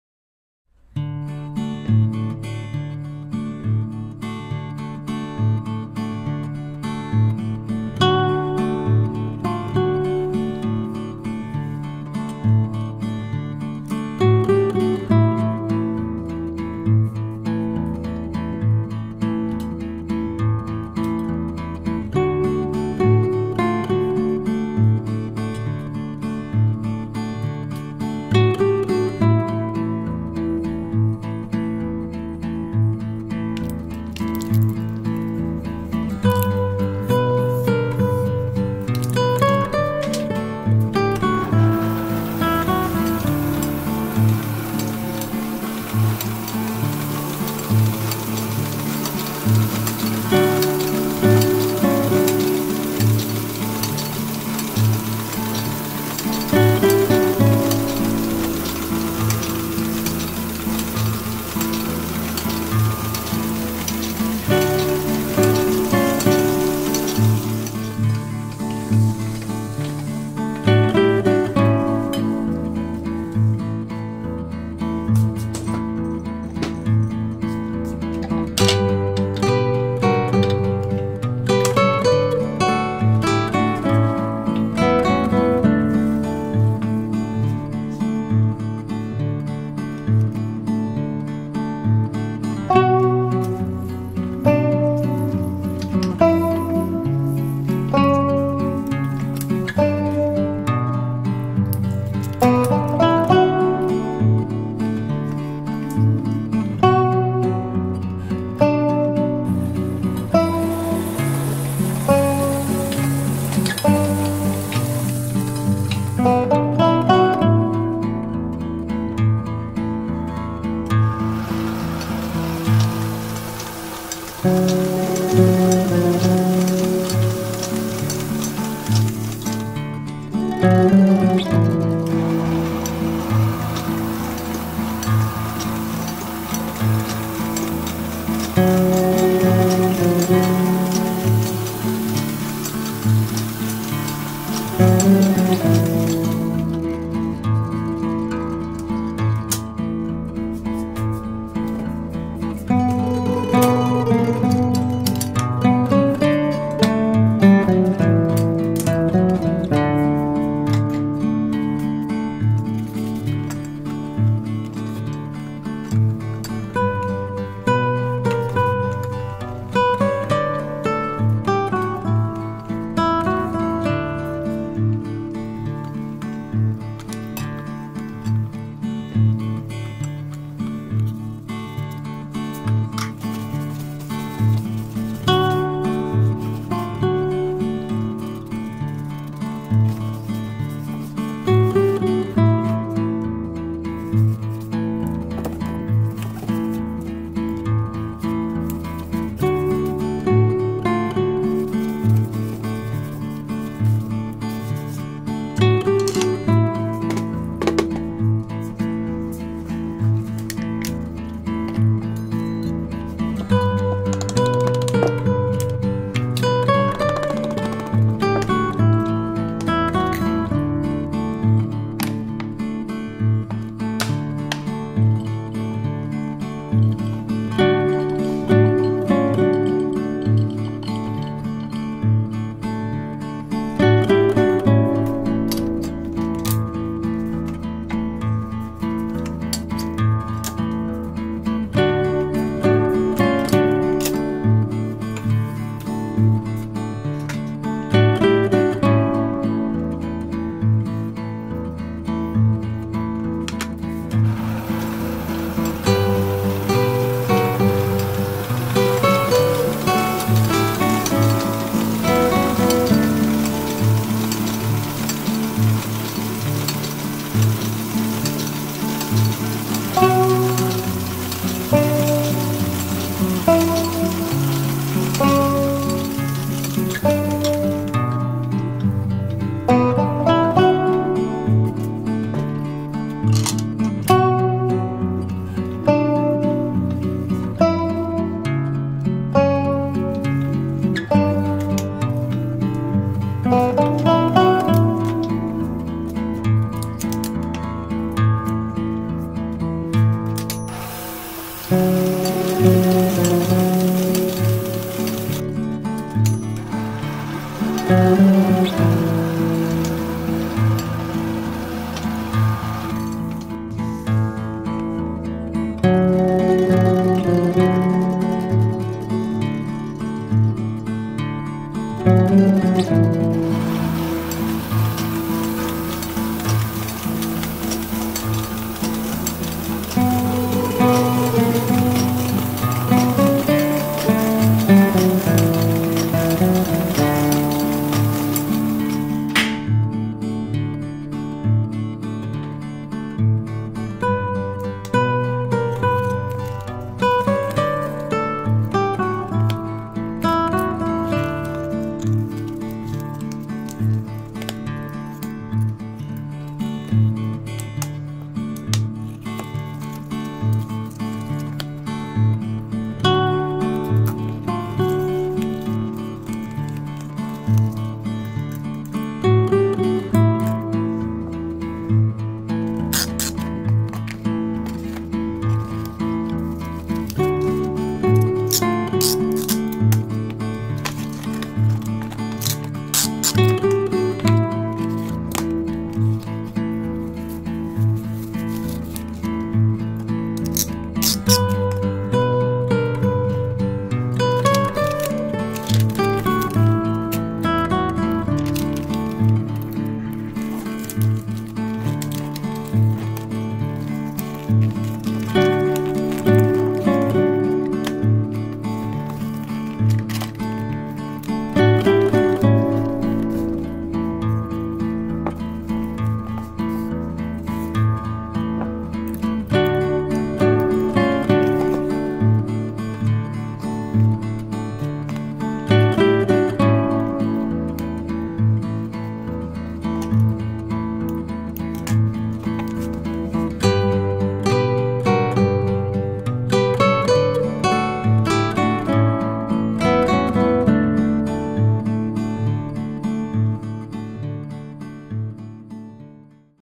Silver + Black printing on letterpress black paper.
레터프레스 검정용지에 은색+검정색 인쇄하기
I Love Letterpress.